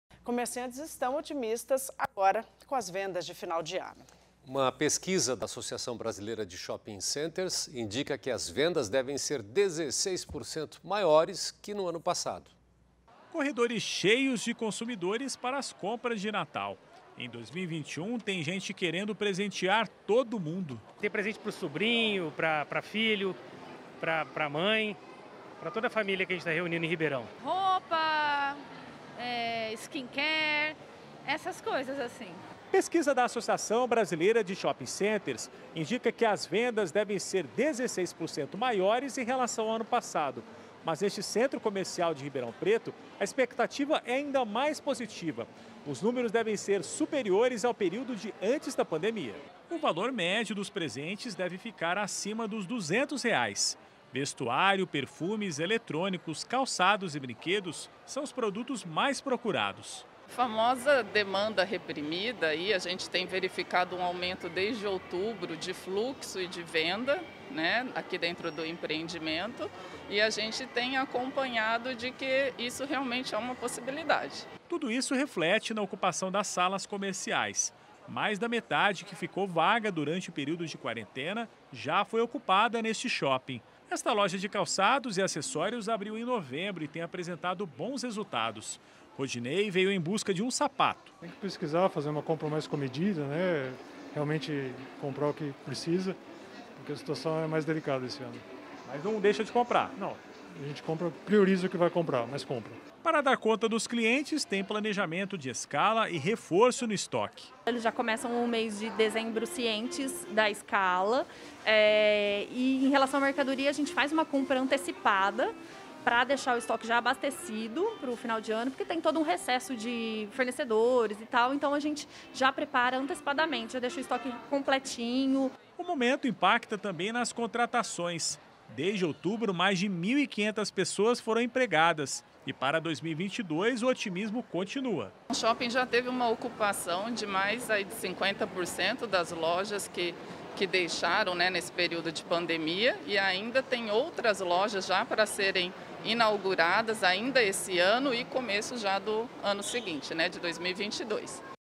Nos shoppings, lojistas acreditam em vendas 16% maiores
Apesar dos prejuízos causados pelas obras paradas em Ribeirão Preto, comerciantes estão otimistas com as vendas de final de ano. Uma pesquisa da Associação Brasileira de Shopping Centers indica que as vendas devem ser 16% maiores que no ano passado. .